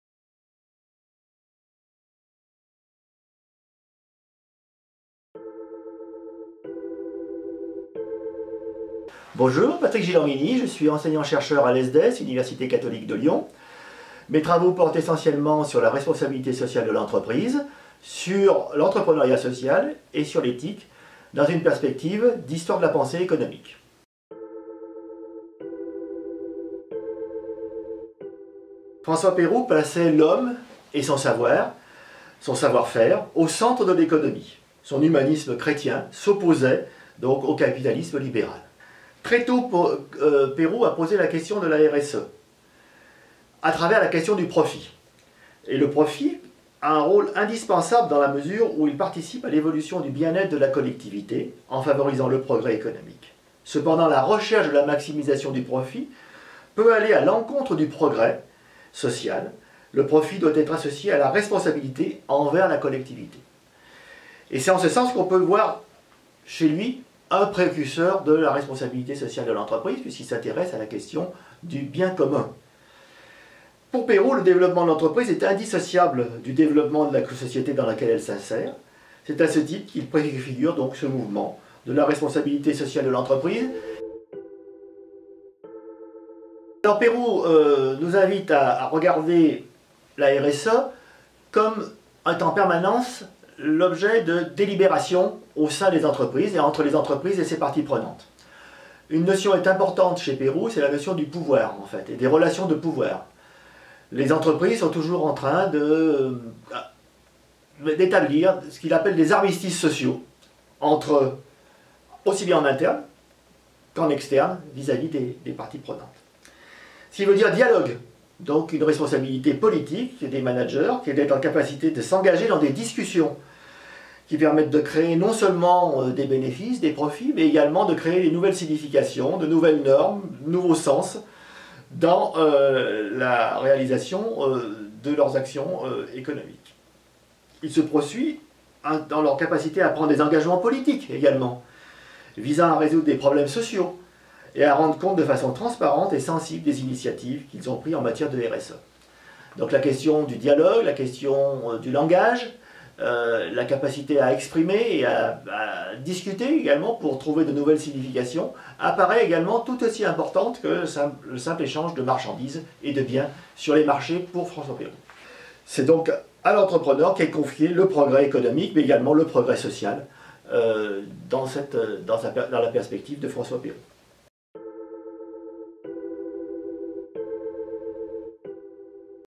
La Responsabilité Sociale des Entreprises - Par Patrick GILORMINI
Patrick GILORMINI, Enseignant-chercheur à l'ESDES nous explique le chapitre "François Perroux : une pensée économique précurseur de la responsabilité sociétale des entreprises ?" qu'il a écrit dans le cadre de réalisation de l'ouvrage "La Responsabilité Sociale des Entreprises" par les enseignants chercheurs de l'ESDES.
Découvrez les informations concernant le Laboratoire de Recherche à l'ESDES Lyon Business School